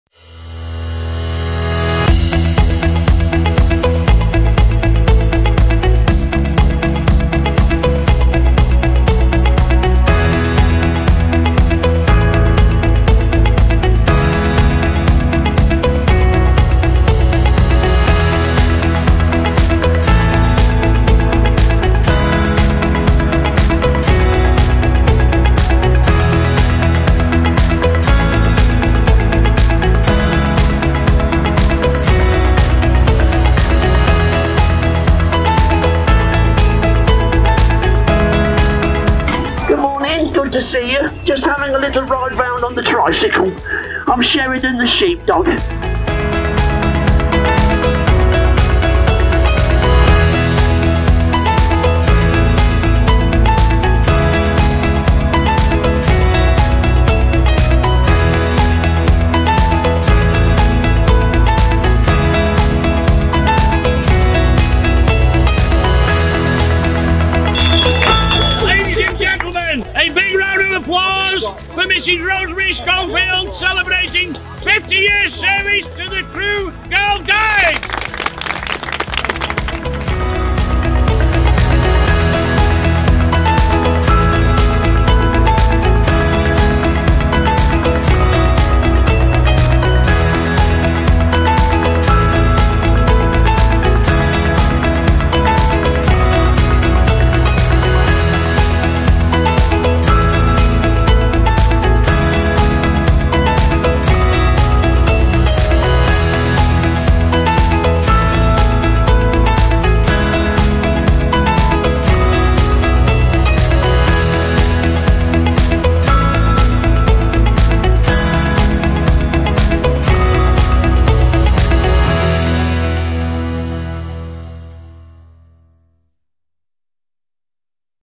Reaseheath College's Family Festival 2019
Take a look of some highlights of our annual Family Festival last year!

Unfortunately, the event was cancelled this year due to the ongoing situation with coronavirus.

We are now focusing on next year and can't wait to be back bigger and better in May 2021 as part of our 100 year celebrations!